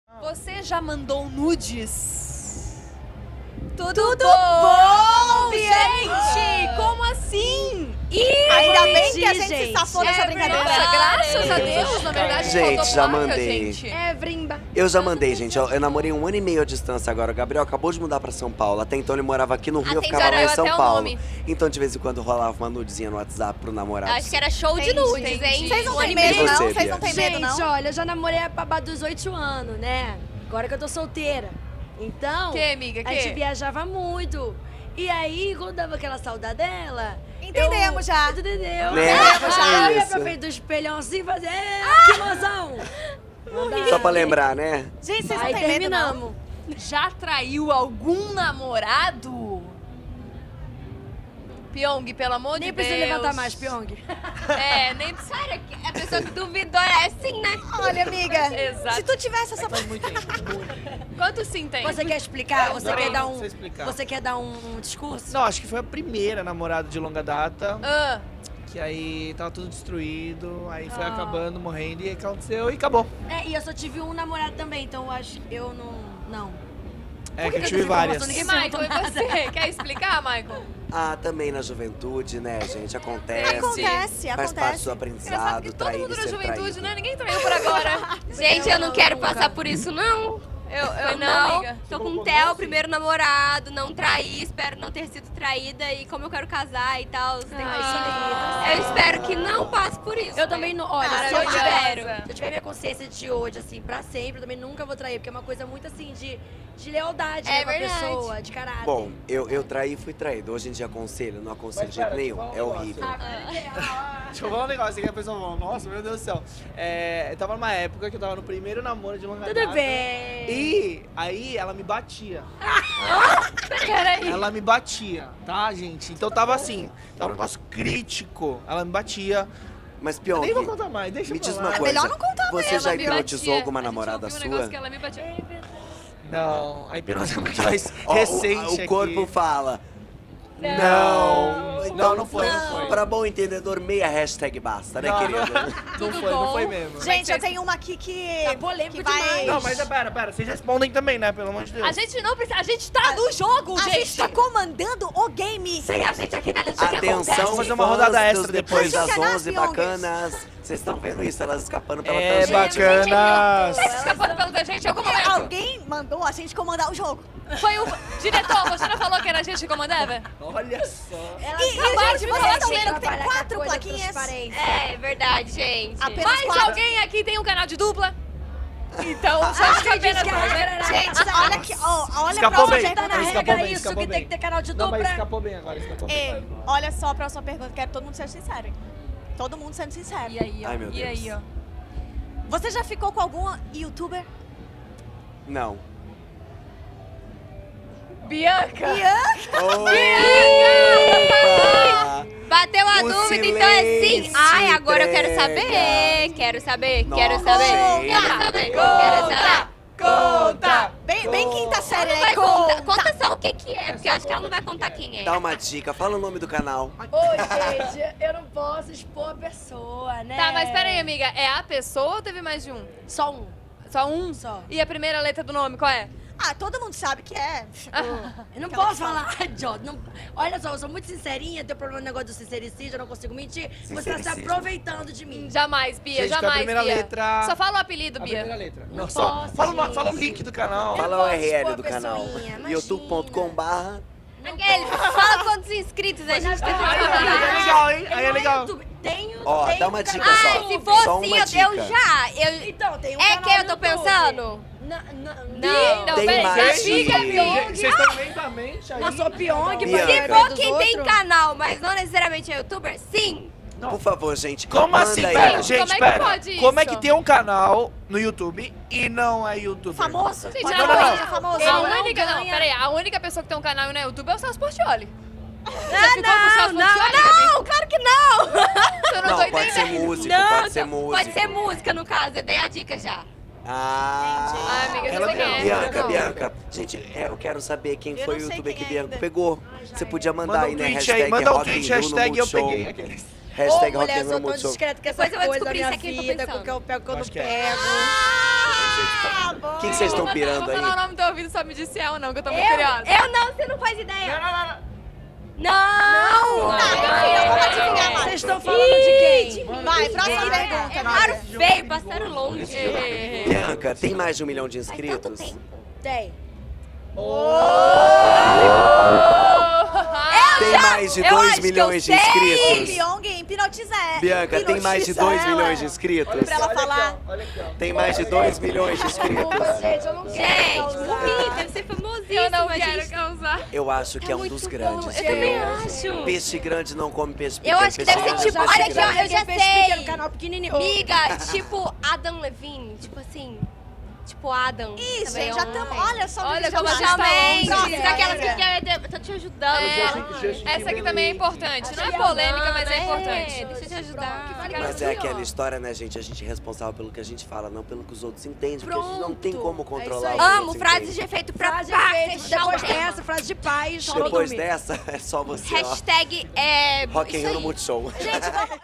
Sim ou não? | Maicon + Boca + Pyong + Luthai + Depois das 11 | Glass Studio | Rock In Rio 2017
Os youtubers Maicon Santini, Pyong, Gabi Luthai, Roca Rosa e Depois das 11 brincam de "sim" ou "não" no Glass Studio.